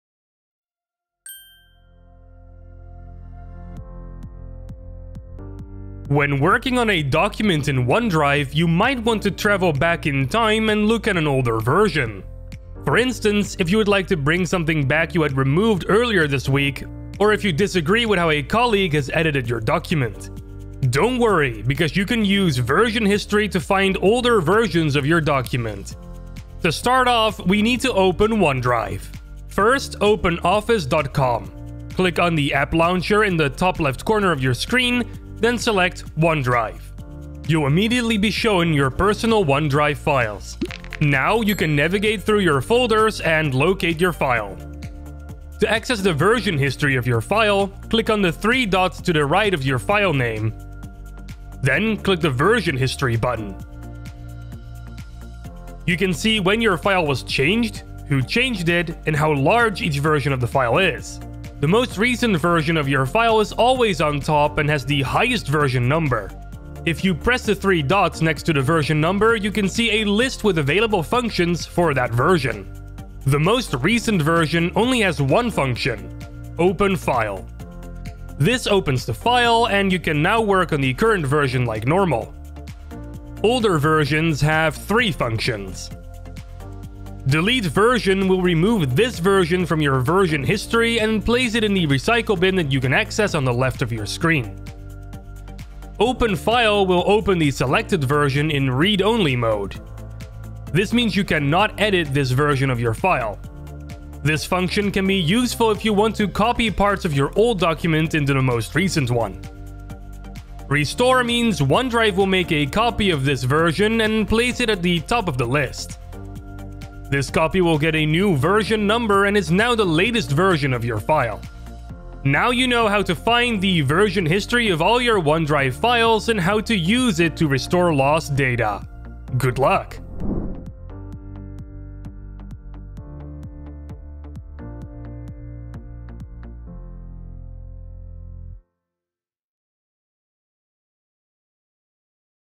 How to use version history in OneDrive
It can be super annoying when a change is made to a document that you didn't approve! Luckily you can use OneDrive version history to find older versions of your document, in this video we will show you how.

Host: Mitch Wensveen
Writer/editor: Pepijn van Setten